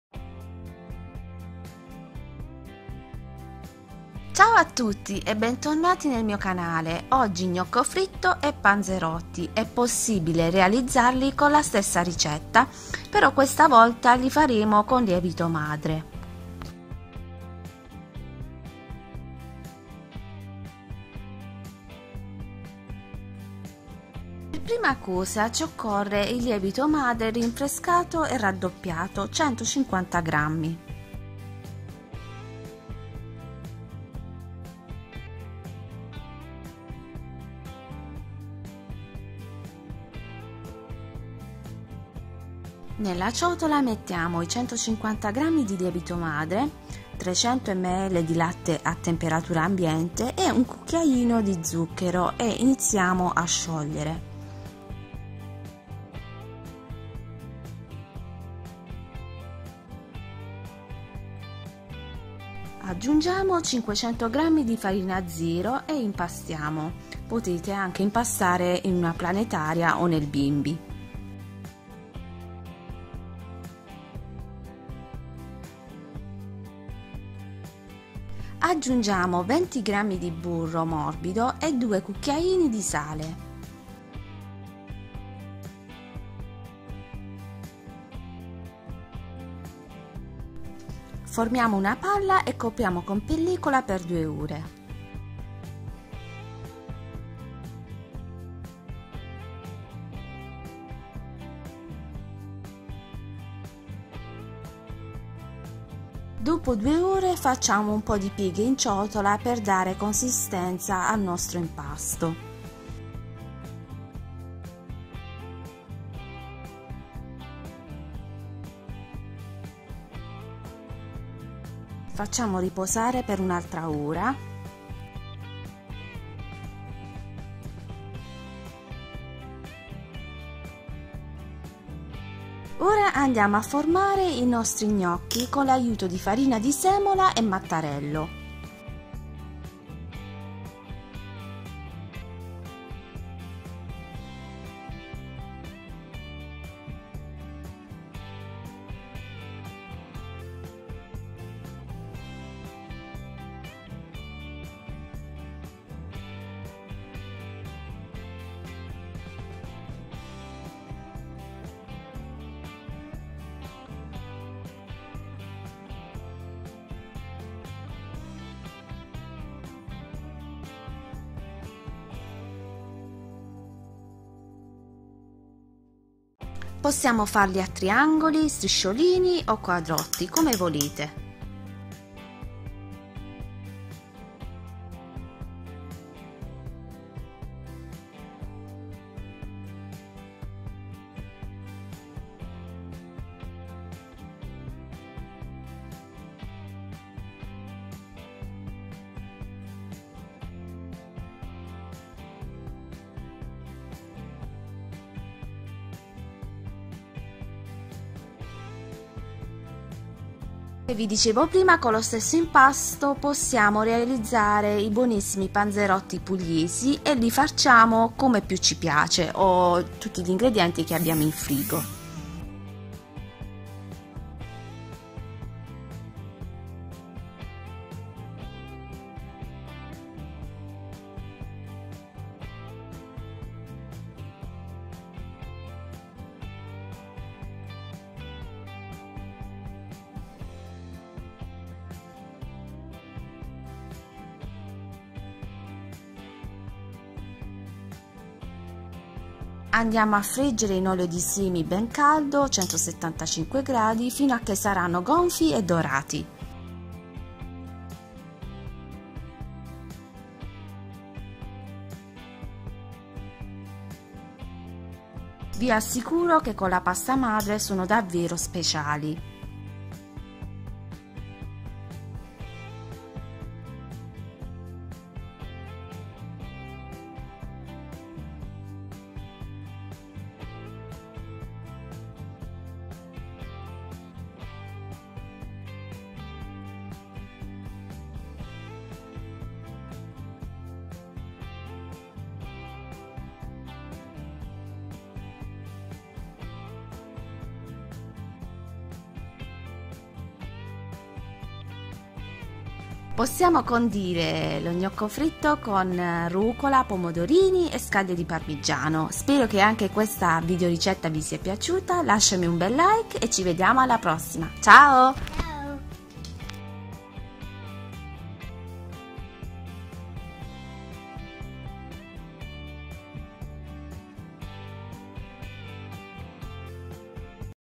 Gnocco fritto e panzerotti con lievito madre - Stesso impasto
Gnocco fritto e panzerotti fritti pugliesi con lievito madre:
300 ml di latte
150 g di lievito madre rinfrescato e raddoppiato
1 cucchiaino di zucchero
500 g di farina 0
20 g di burro morbido
2 cucchiaini di sale